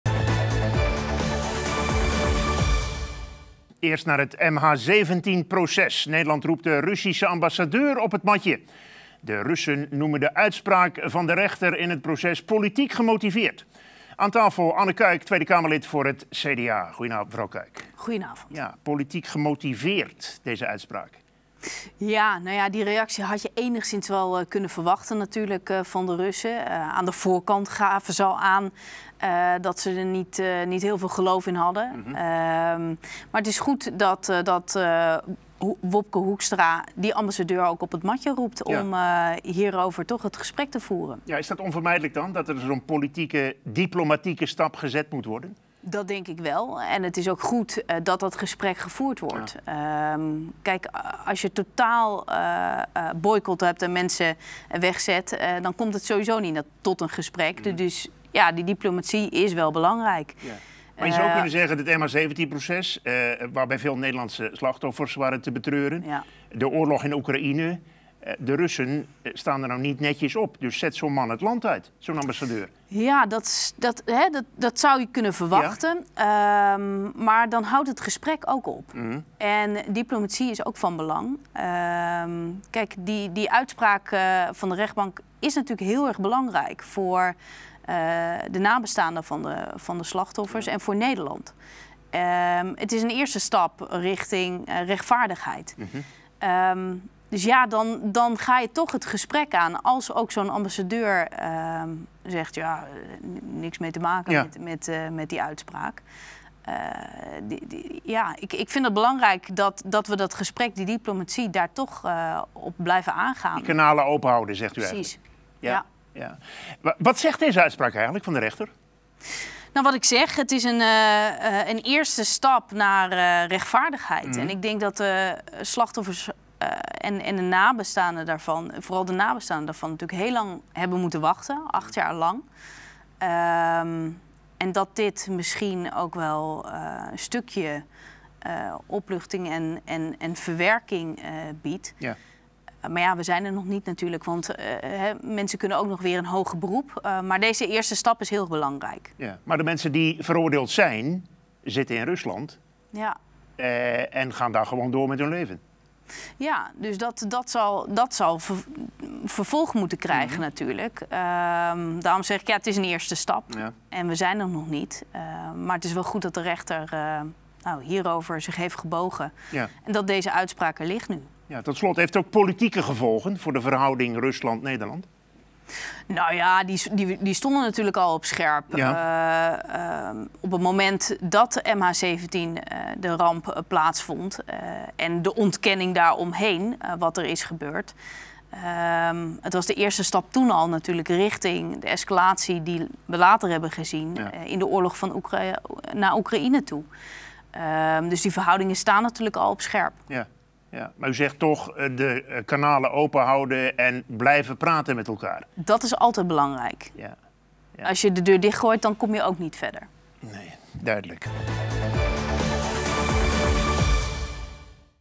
Uitgelicht! 18 november 2022 - Anne Kuik (CDA) over kritiek Russische ambassadeur op MH17-uitspraak
Nederland roept de Russische ambassadeur van op het matje. De Russen noemden de uitspraak van de rechter politiek gemotiveerd. Minister Hoekstra noemt de reactie volstrekt verwerpelijk. De verdachten van de MH17 verblijven momenteel in Rusland. Hoe groot is de kans dat ze ooit in Nederland achter de tralies verdwijnen?